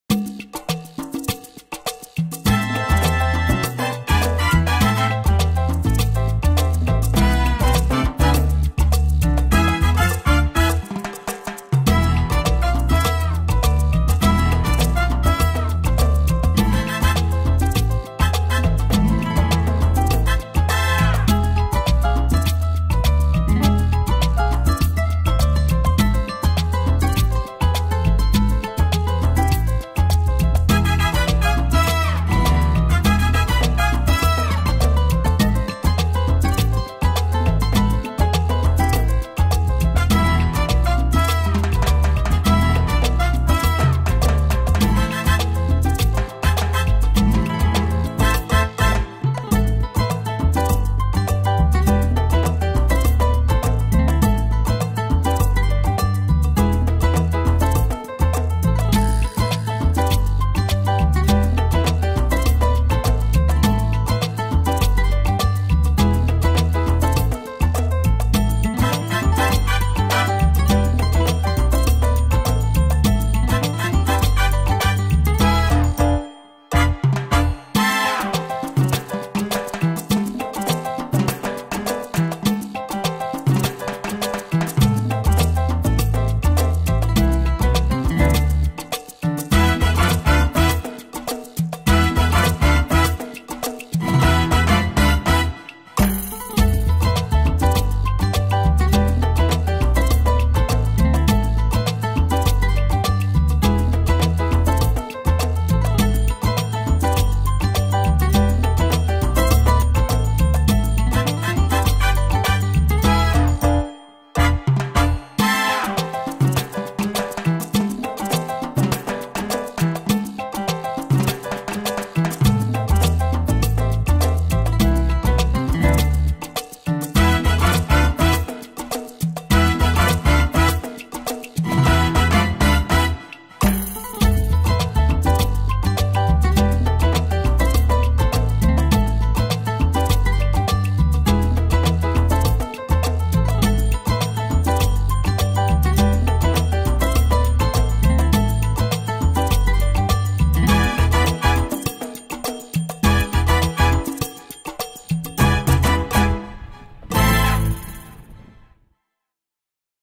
"Little Corn Island, Nicaragua. Boat Tour around the island, from Yemaya Island Hideaway & Spa"
A boat tour around Little Corn Island reveals  hidden secluded beaches, rocky cliffs, small boutique hotels, a small "town", all framed against a tropical  background of tall coconut trees...and surrounded by clear pristine water perfect for diving or snorkeling.  Starting and ending at Yemaya Island Hideaway & Spa